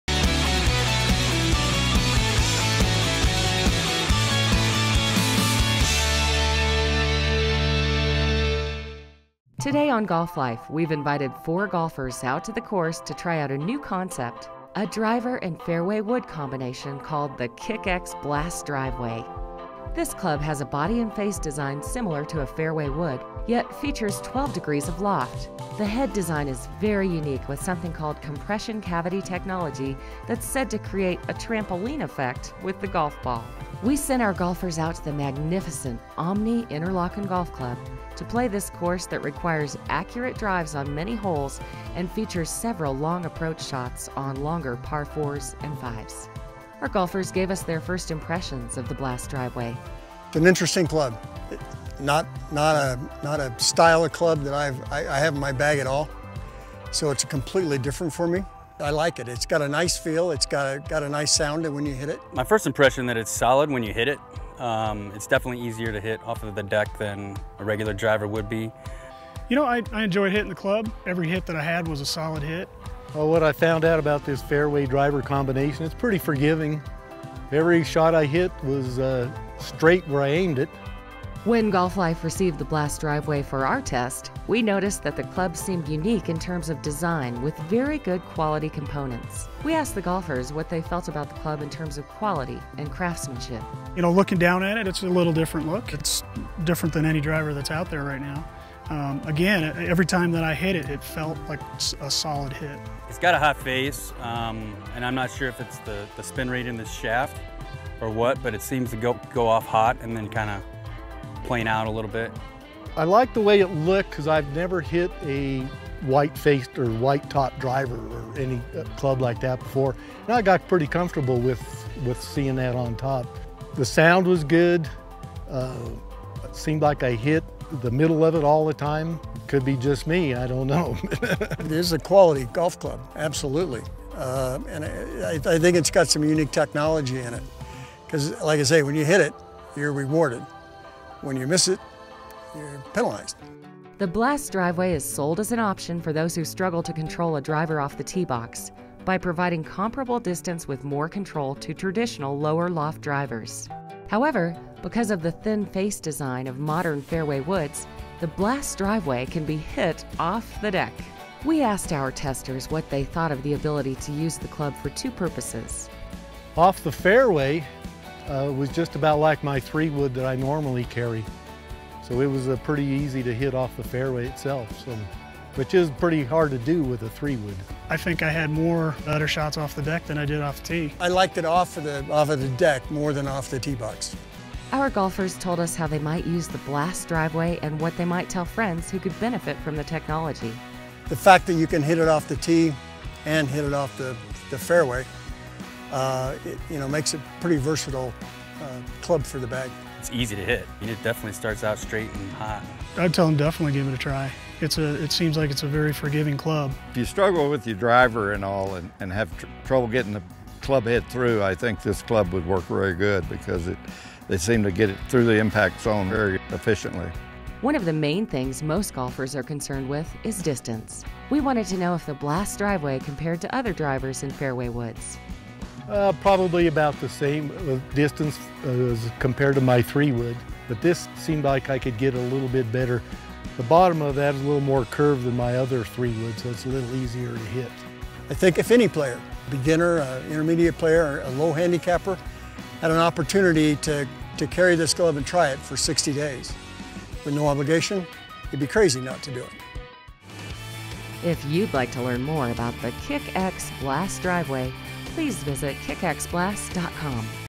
Kick X Blast Driveway
Golf Life learned more about a Driver and Fairway wood combination called the Kick X Blast Driveway.

This club has a body and face design similar to a fairway wood, yet features 12 degrees of loft. The head design is very unique with something called compression cavity technology that is said to create a trampoline effect with the golf ball.

Try the Kick X Blast Driveway today Absolutely FREE for 60 days!!!